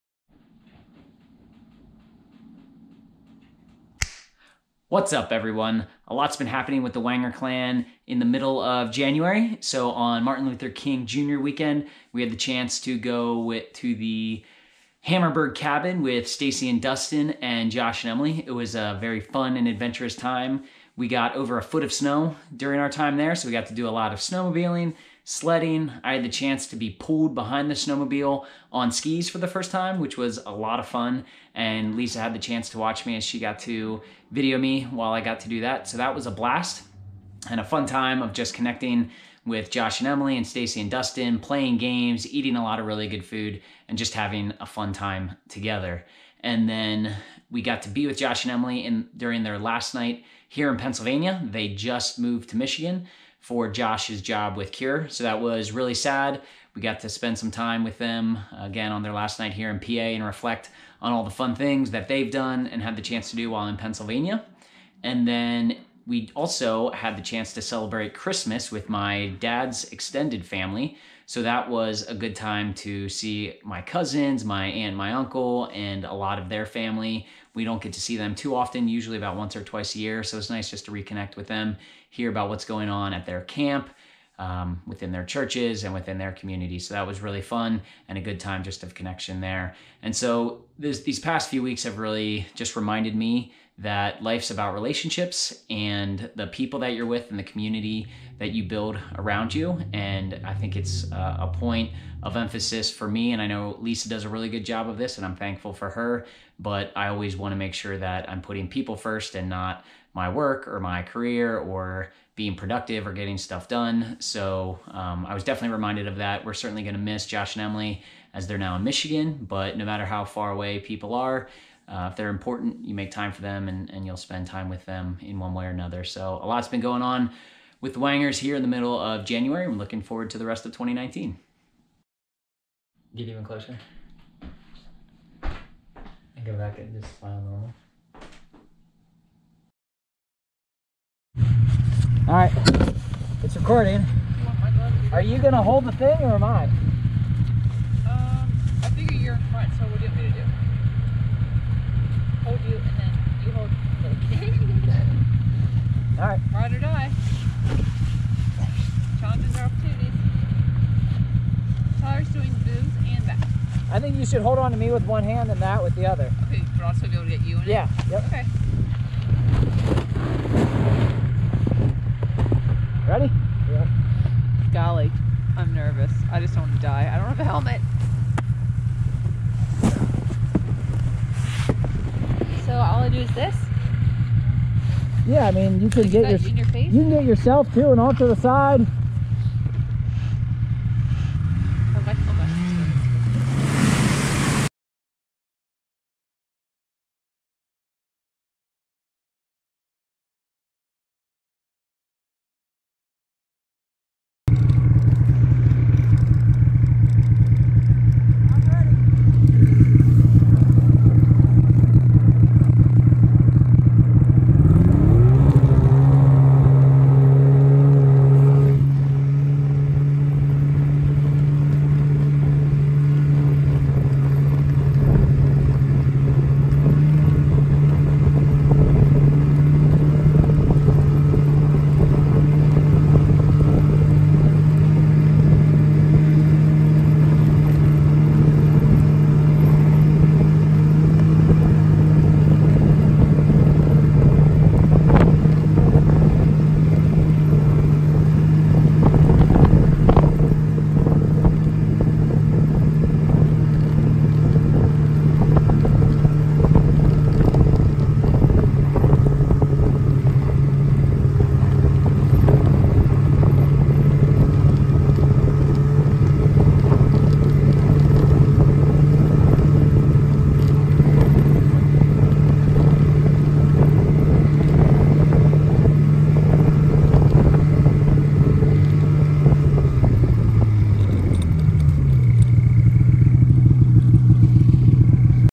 January 2019 - Cabin Weekend & Wenger Extended Family Christmas
In the middle and end of January, we went to the Hammarberg Cabin with Stacey, Dustin, Josh & Emily (it got down to -12 degrees the one morning) and then we celebrated Christmas with my Dad's extended family.